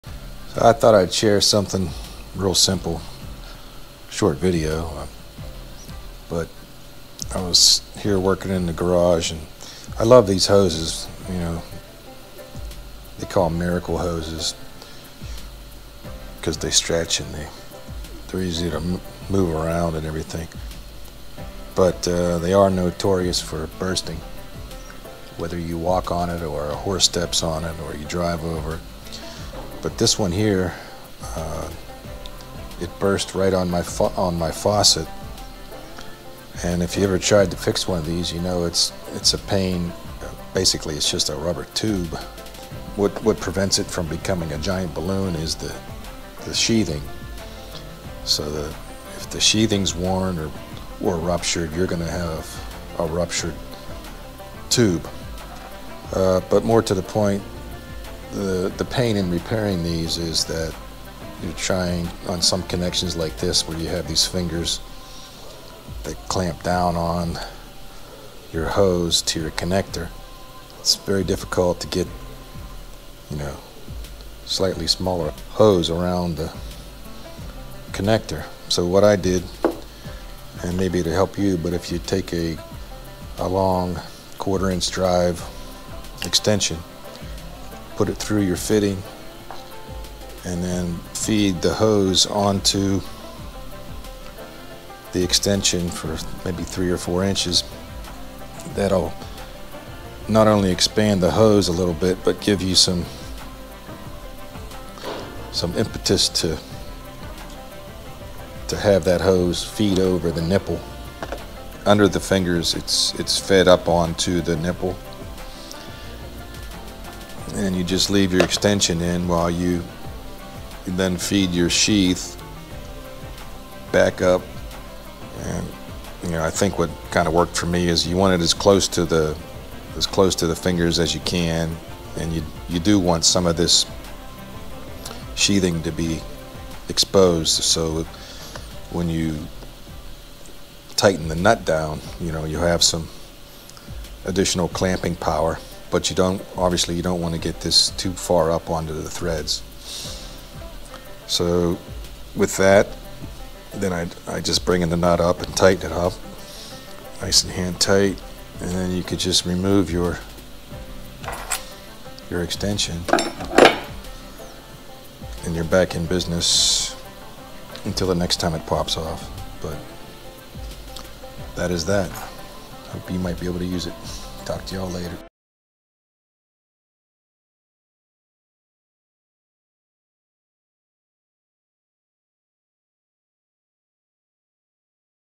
Can You Fix a Pocket Hose? 💦 | Fabrication Philippines | Ep. 01 | TORINO
Sharing my method to easily repair a hose end on a miracle or pocket hose.